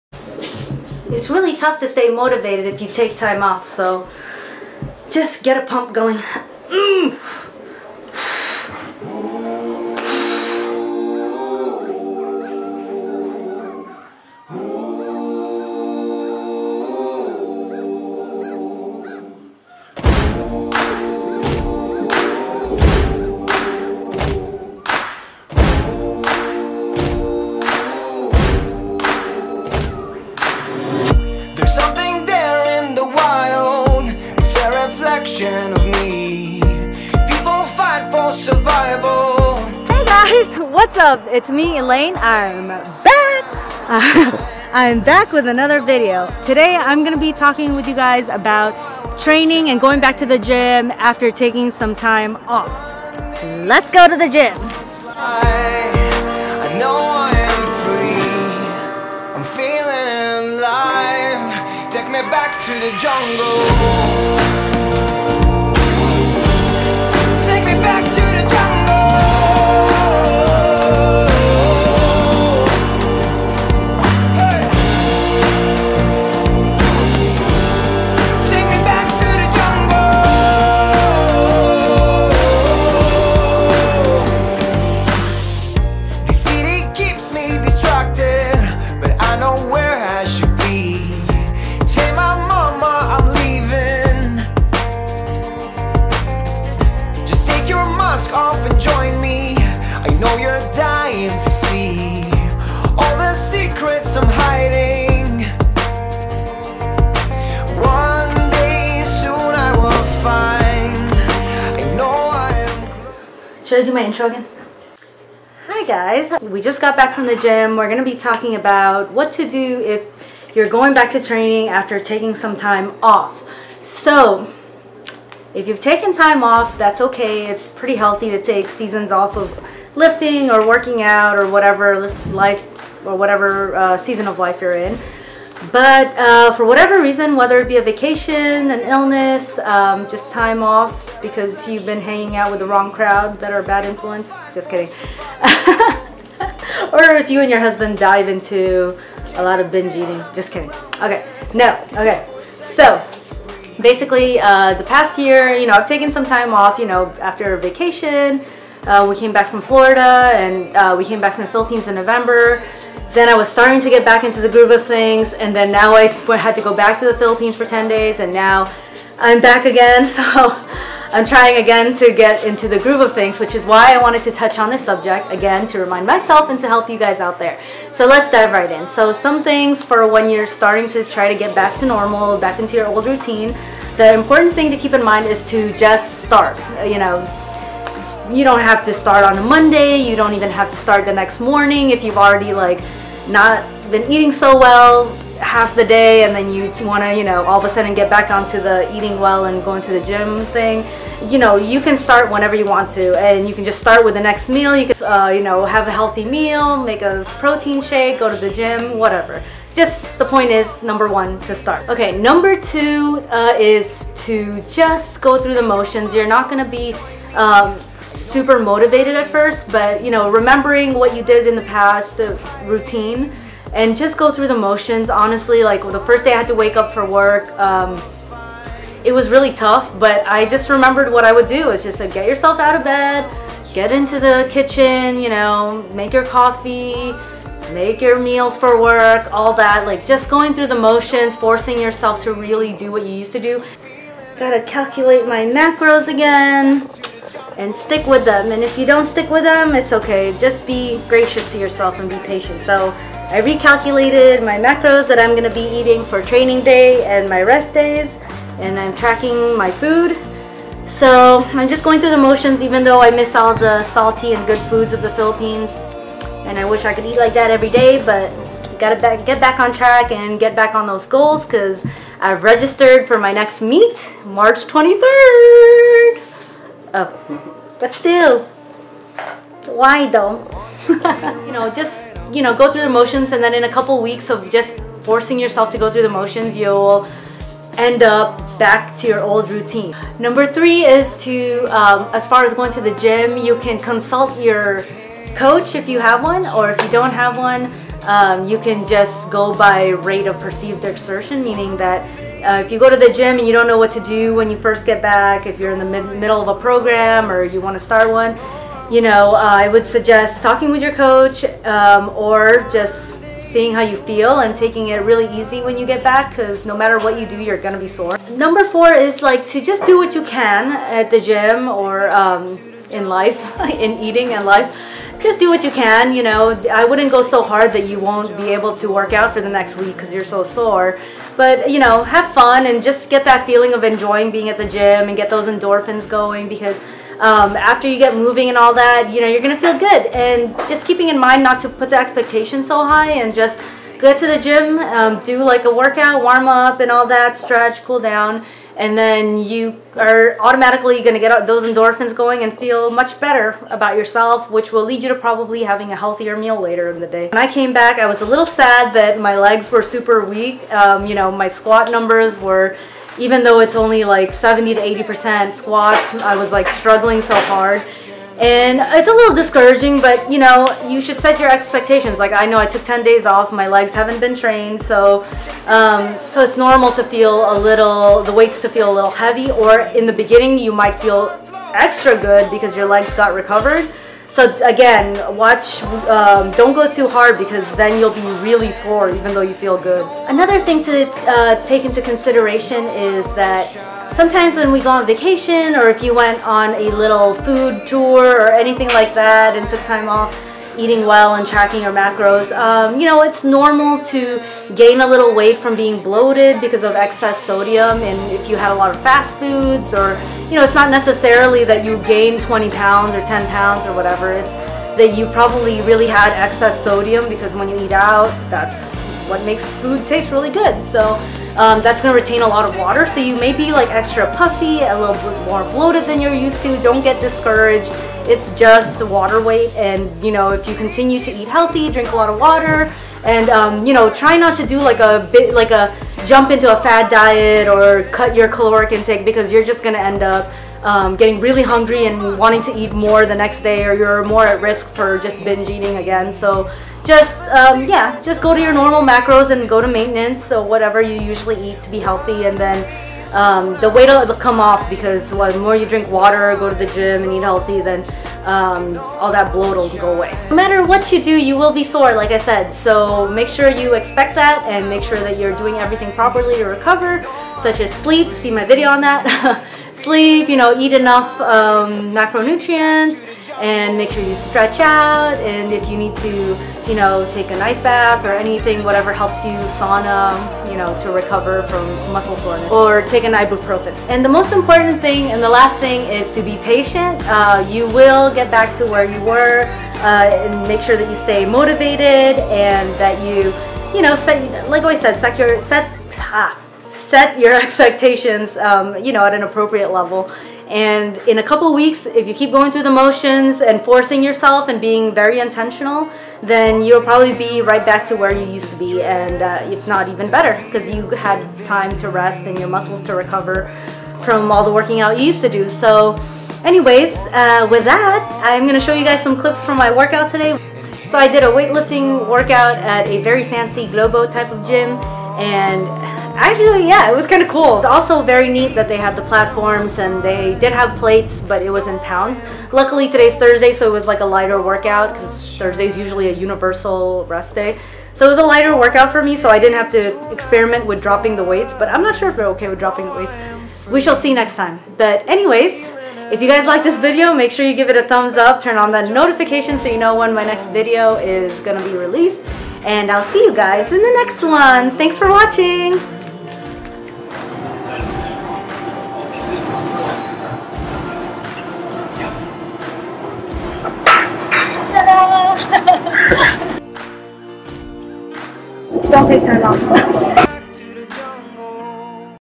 HOW To Train After Taking Time Off VLOG
Hi guys! I took a lot of time off recently and now trying to get back into my training and eating routine. I know many can relate. In this video I go over my strategies for getting back the gains or at least on the road to it. I also have my training clips from Thursday at Equinox in Palo Alto, Ca.